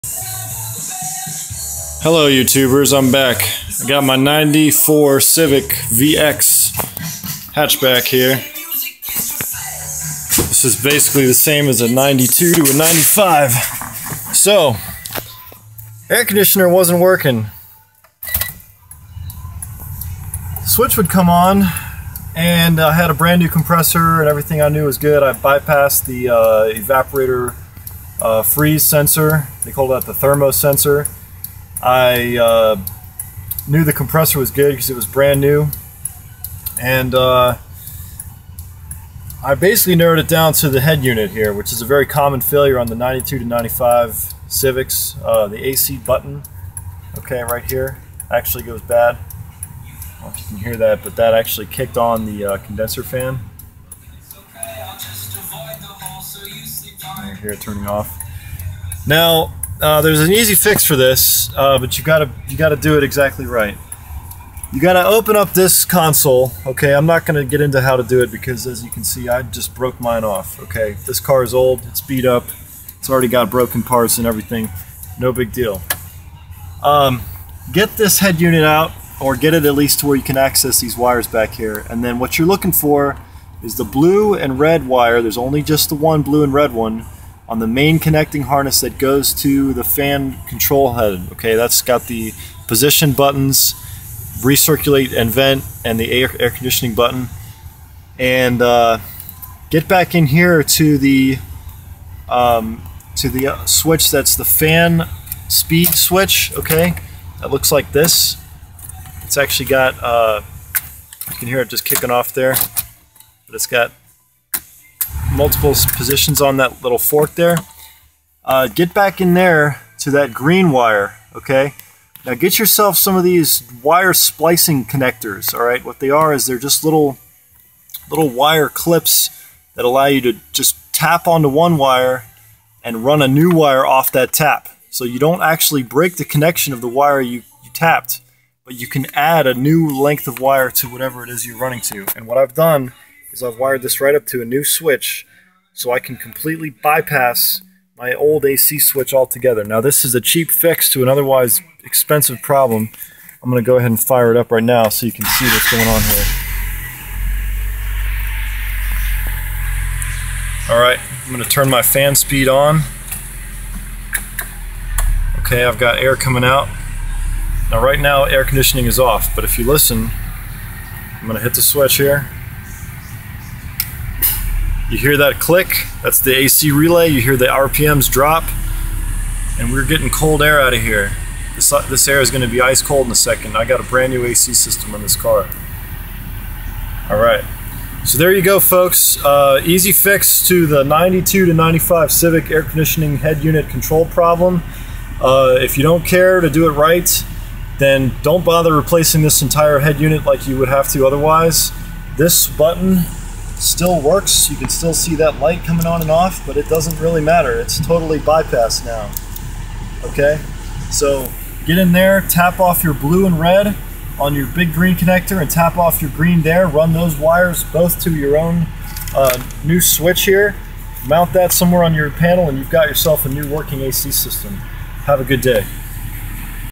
92-95 Civic A/C Head Unit Repair
UPDATE: Resoldering the points on the main board should also solve the problem.

Repaired the broken AC button on my 94 Civic VX hatchback.  As it turns out, some of the solder joints on the control head circuit board were loose, and this repair could have been done simply by resoldering all the joints on the control head circuit board, which I later did.  Shortly after this video was made I discovered that my new switch still wasn't working well, and I deduced that bad connections on the board were the culprit.  If you do decide to run a bypass switch, you should install a diode in series with the switch, to mimic the original board and prevent voltage back spikes.